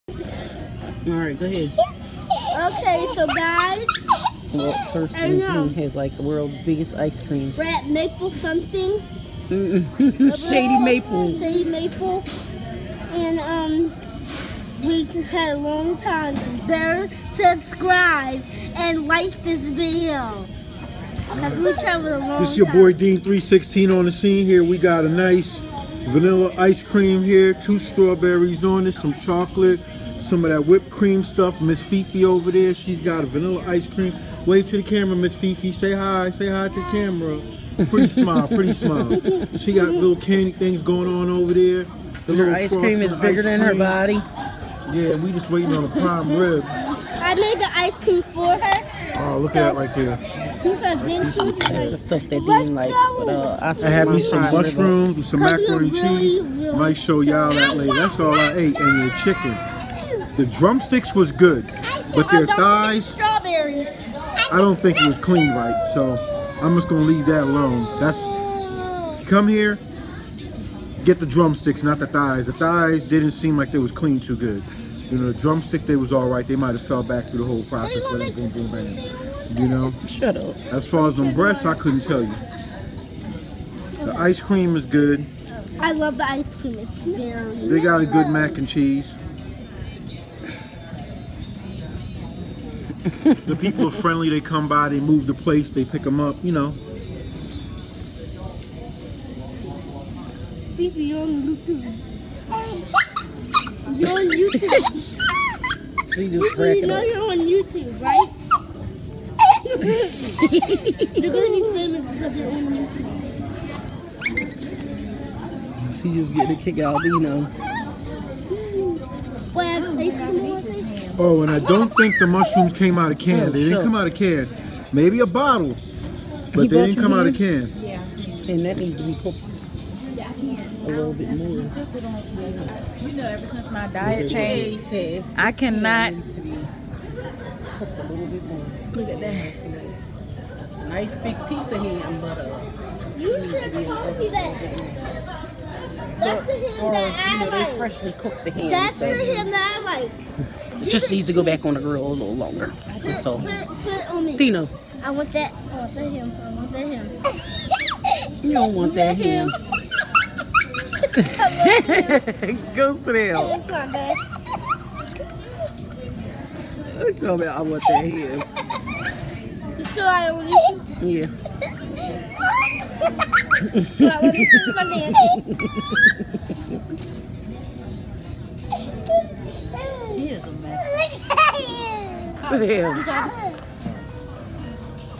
Shady Maple camp ground Amish Country a beautiful place Located in Lancaster PA. BF and Smorgasbord
It was an all u can eat Buffet but nobody was really that Hungry 😋 The prime Rib Tho🤗😜😜